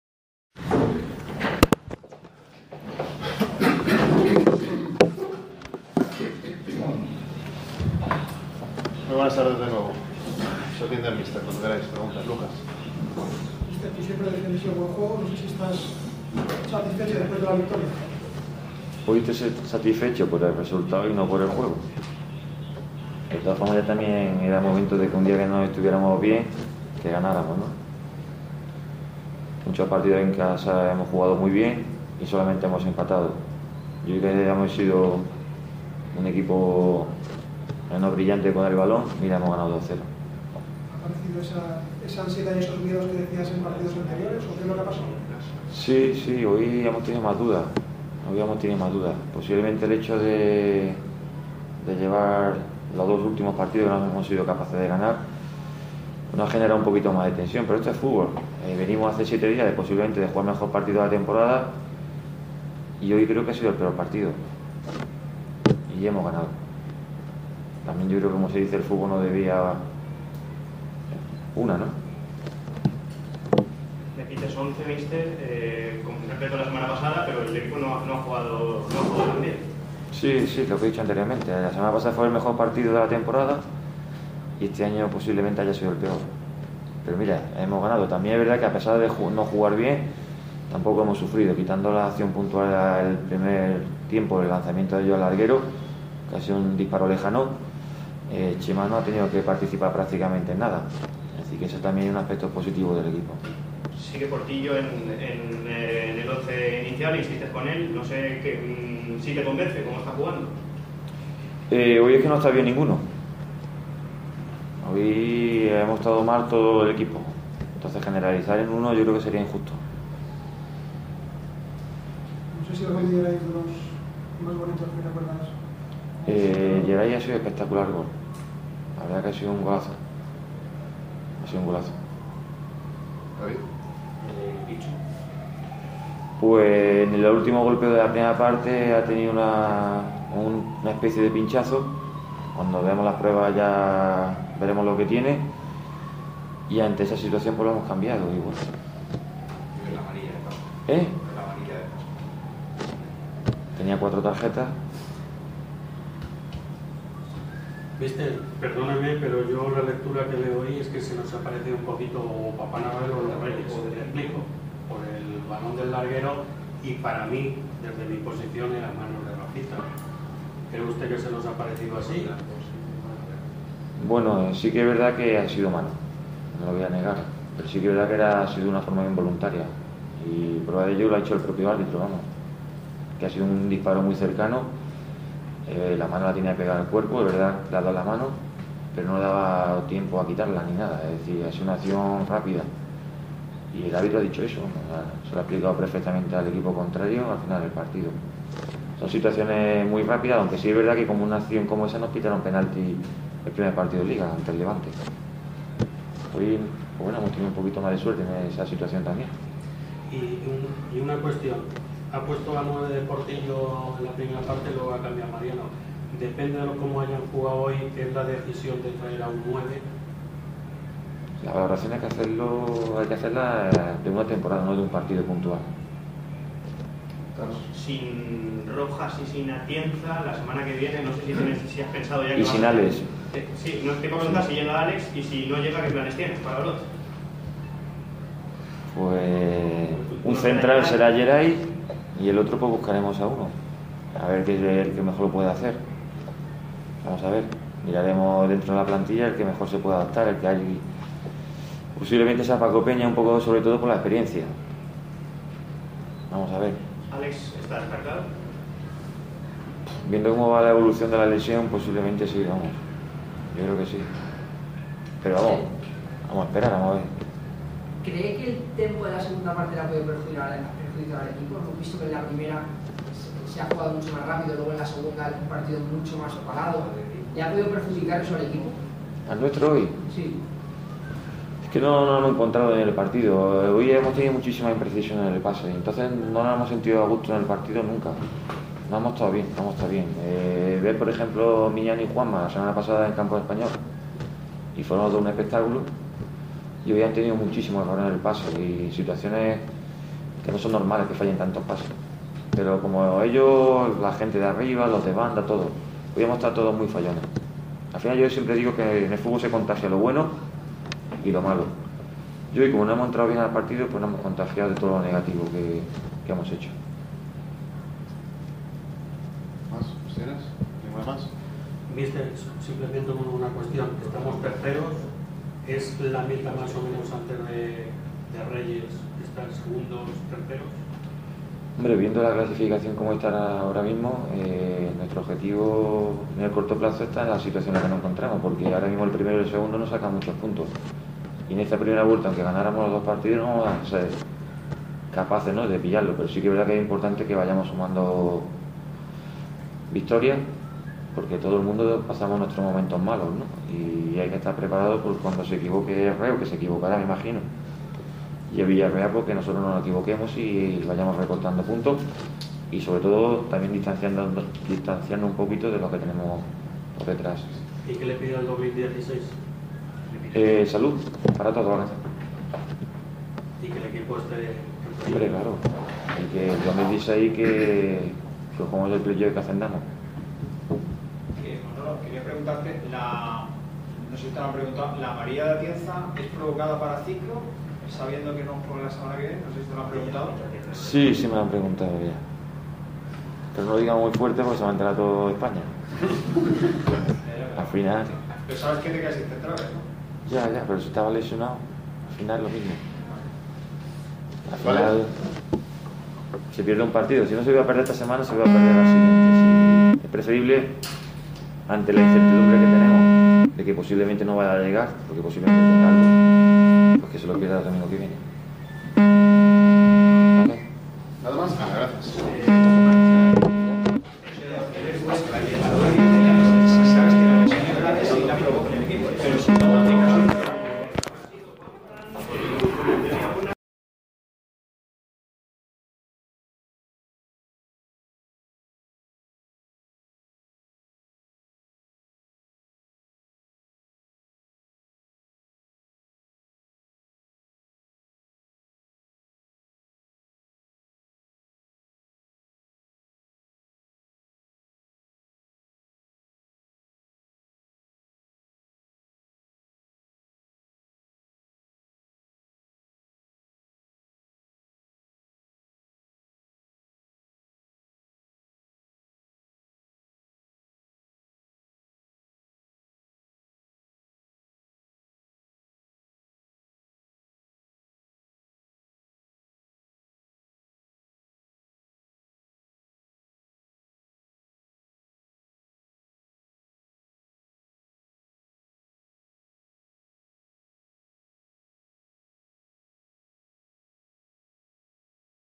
Rueda de prensa de Manolo Herrero Hecules - Hospitalet
Rueda de prensa de Manolo Herrero tras el partido Hércules - Hospitalet el día 13/12/15.